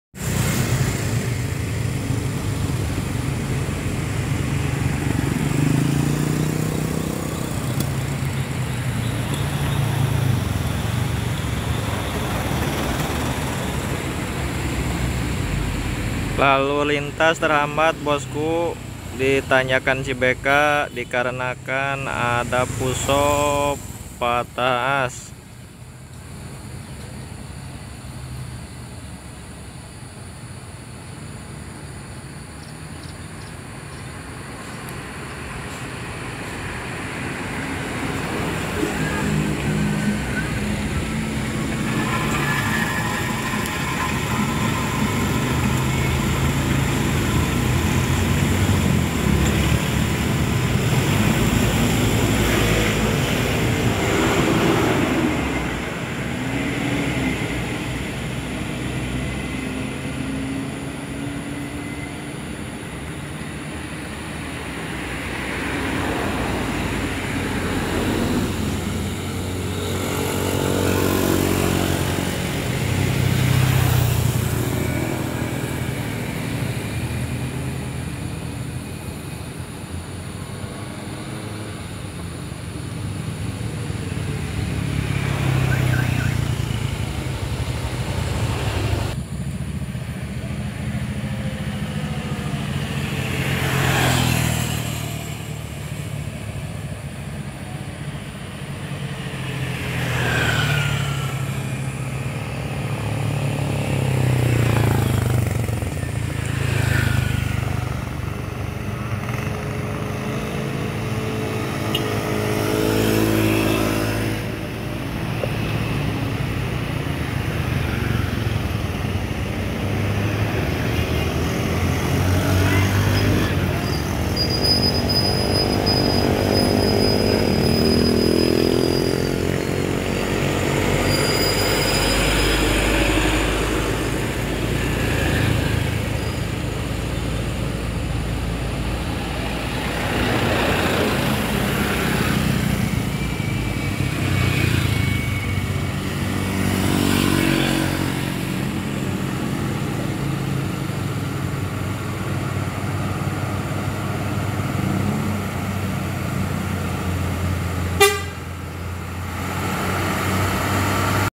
truk fuso patah AS ditanjakan cibeka
lalulintas terhambat ditanjakan
cibeka ciamis dikarenakan ada truk fuso patah as